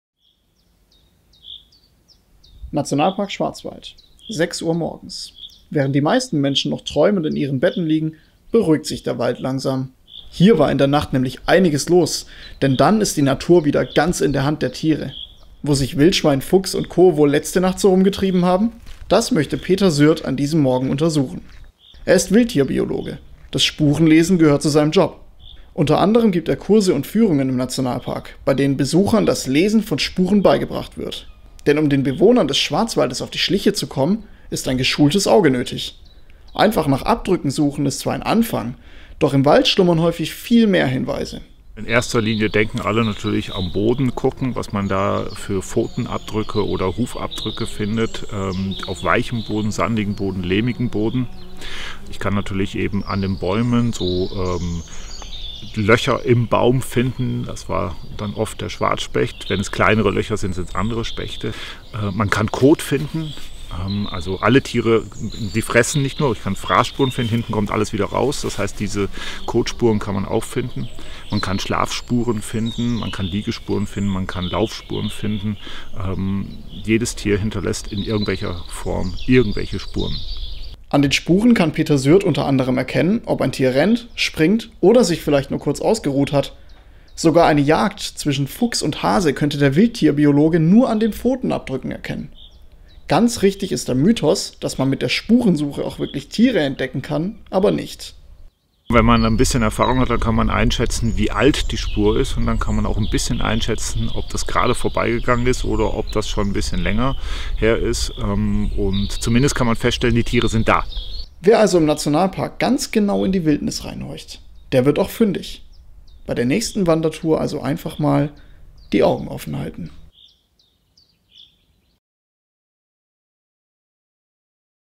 Nationalpark Schwarzwald Blog: Fährtenlesen für Anfänger
Welche Tiere wohl nachts unbemerkt durch den Nationalpark streifen? Nur mit einem geschulten Auge lässt sich das anhand ihrer Spuren, die sie hinterlassen, erkennen. Zusammen mit dem Wildtierbiologen Peter Sürth macht sich Blogautor Oliver auf Spurensuche im Nationalpark.